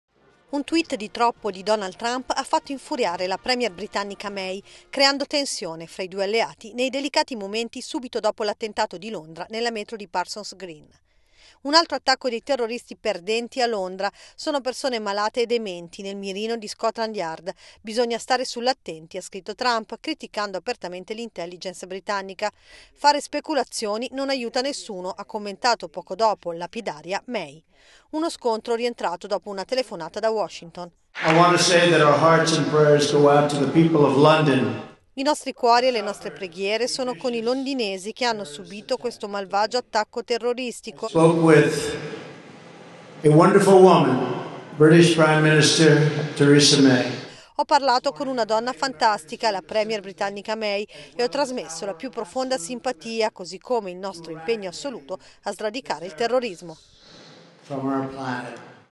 Scontro fra Trump e May dopo l'attacco in metro a Londra
Milano (askanews) - Un tweet di troppo di Donald Trump ha fatto infuriare la premier britannica May creando tensione fra di due alleati nei delicati momenti subito dopo l'attentato di Londra nella metro di Parsons Green.
"Un altro attacco di terroristi perdenti a Londra. Sono persone malate e dementi nel mirino di Scotland yard. Bisogna stare sull'attenti" ha scritto Trump criticando apertamente l'intelligence. "Fare speculazioni non aiuta nessuno", ha commentato poco dopo, lapidaria, May.
Uno scontro rientrato dopo una telefonata da Washington. "I nostri cuori e le nostre preghiere sono con i londinesi che hanno subito questo malvagio attacco terroristico. Ho parlato con una donna fantastica, la premier britannica May e ho trasmesso la più profonda simpatia così come il nostro impegno assoluto a sradicaare il terrorismo".